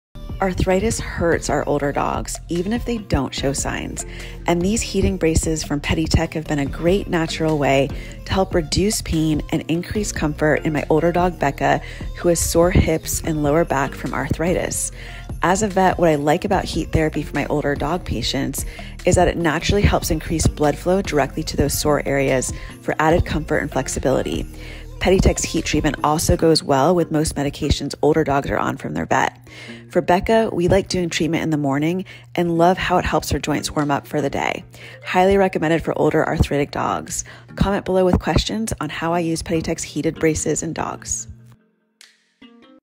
A Simple Way to Help Dogs with Arthritis - From Senior Dog Veterinarian
Arthritis HURTS our older dogs, even if they don't show signs. And one simple way to help reduce pain in our older dogs is through PetieTec's heated therapy braces directly increase blood flow and comfort in those areas of chronic pain. As a vet, it's a great addition for older arthritic dogs because this modality is natural and easily combined with most vet-recommended medications older dogs may already be on.

PetieTec heated therapy braces charge through a USB port and are really easy to apply to our dogs. They have different braces for the neck, back, front legs and back legs and the braces are soft and comfortable. Most dogs really seem to enjoy their heat therapy time. For Becca, we notice that she moves better the days she uses it.